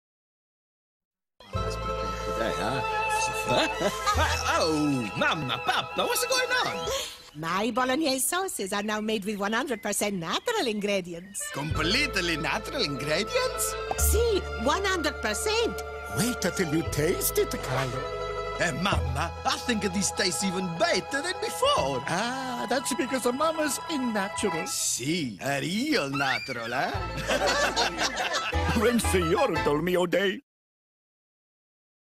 dolmio as in nature intended 2011 advert
mama papa what’sa going on my bolognese sauce is from 100% natural ingredients yes 100%